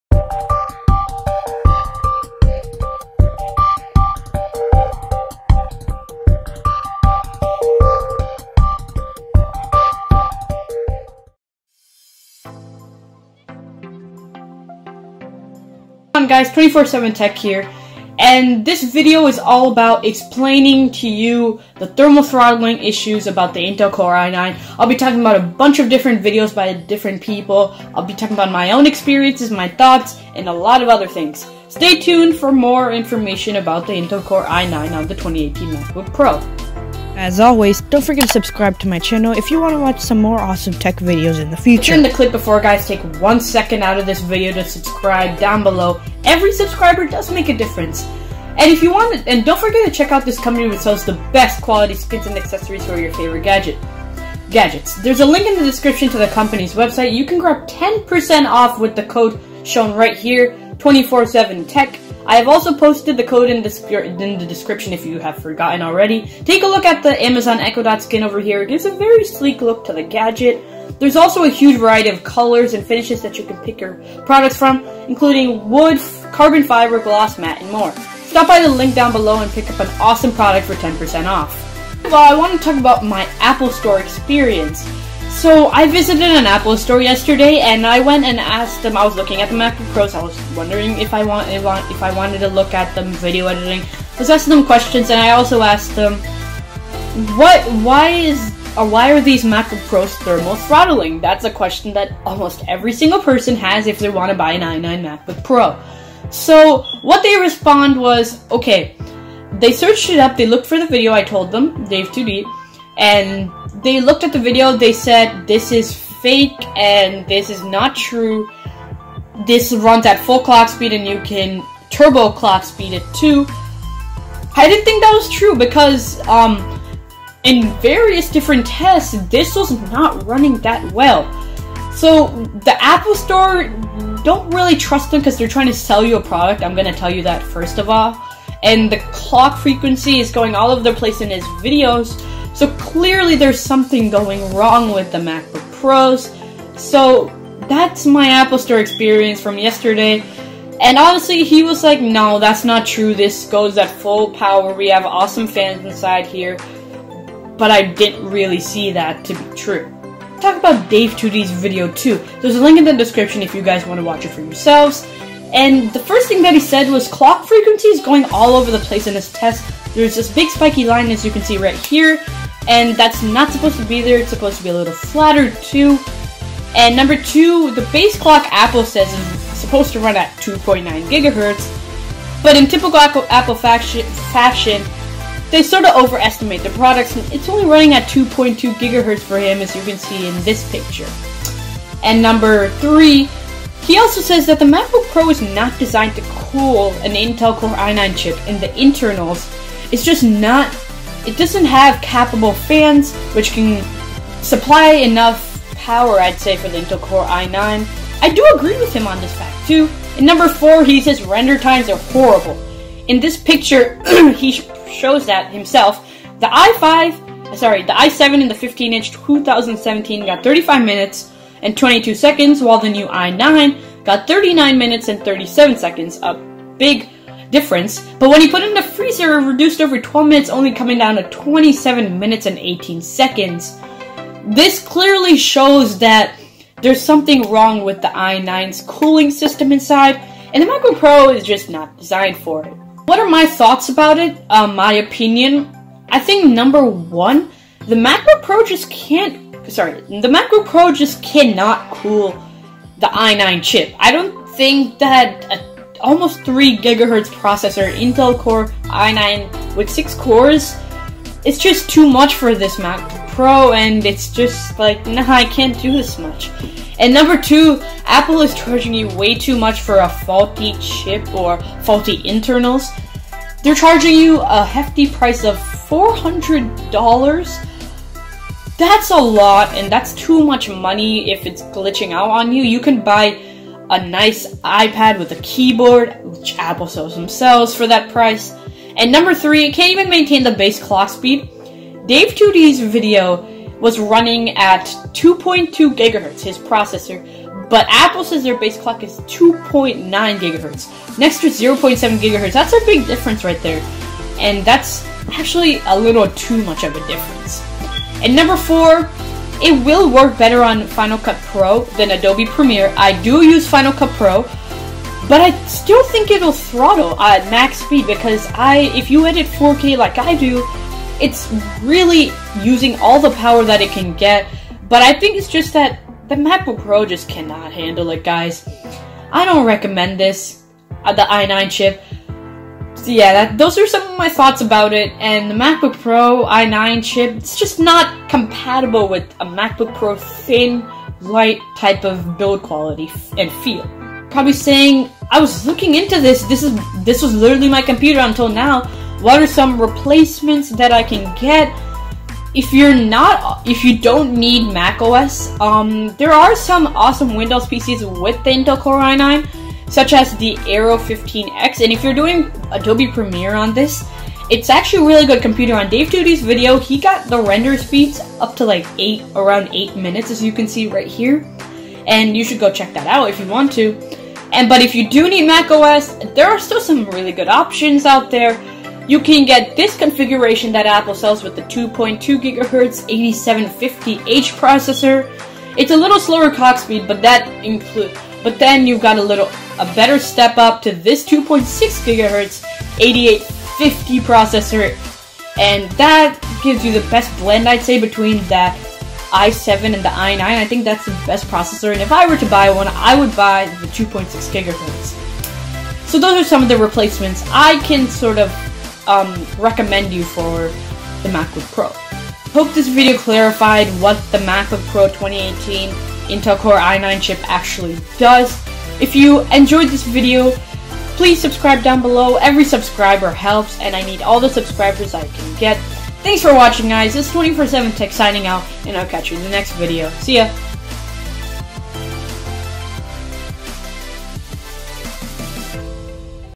2018 MacBook Pro i9 THERMAL THROTTLING- Explained!
If you want to buy awesome Skins and Accessories for your Gadgets, visit GadgetWraps:

The Core i9 is a very capable processor, if it's equipped with the right internals, which this 2018 MacBook Pro clearly doesn't have.

A QUICK BIO: I’m a kid who is making some AWESOME, quality tech reviews about the latest and trending products!

Credit: Dave Lee
Credit: Gadgetwraps
Credit: Apple